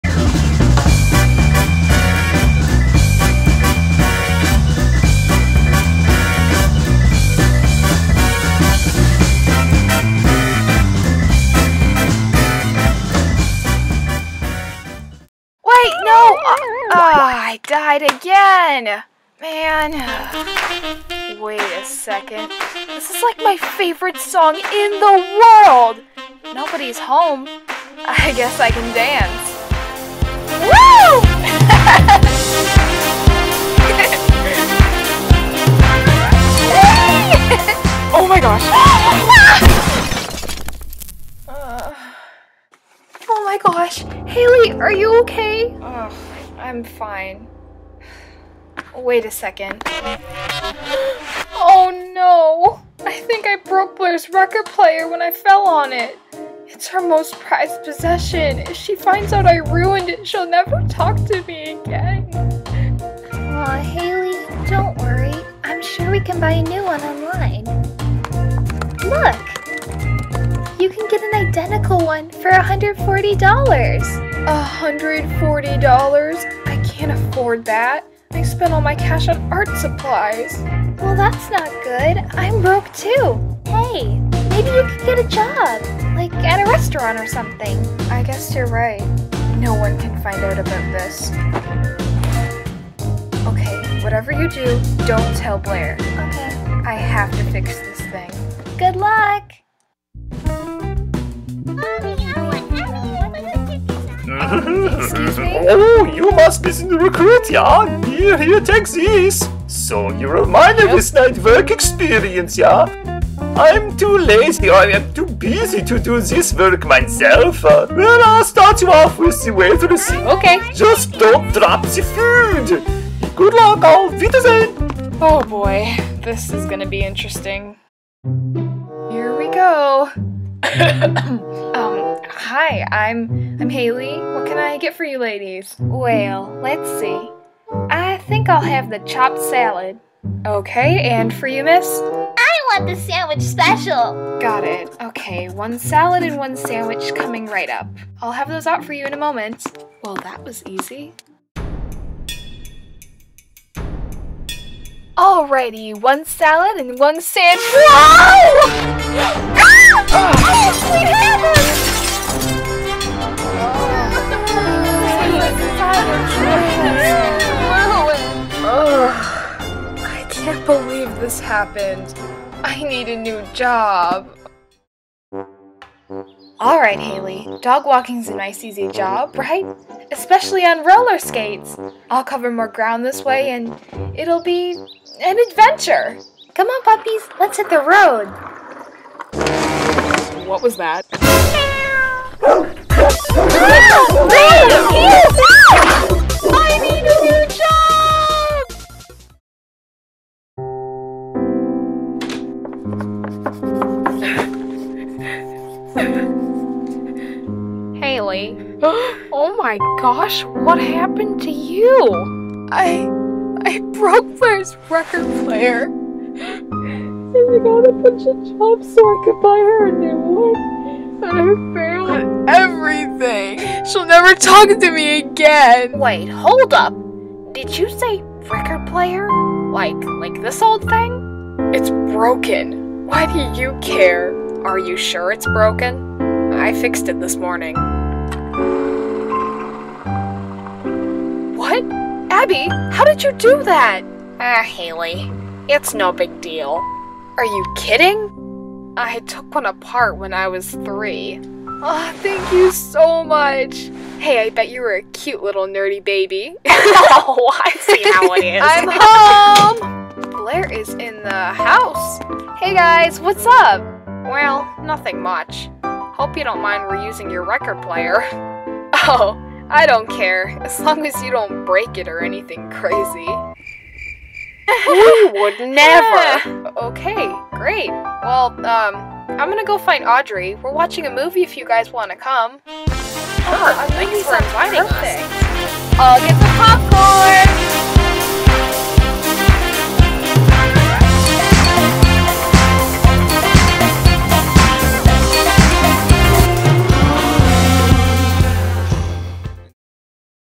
"I Need a New Job!!" ~ AGSM Summer Movie american girl doll stop motion | White Fox Stopmotion
Hailey accidentally breaks Blair's most prized possession- can she find a way to fix things before her sister gets home?!

Ahh finally a new stopmotion!! I'm pretty happy with how this one came out (considering it took two months to make!) and I hope you enjoy watching it as much as I enjoyed making it! ♡ -Emmy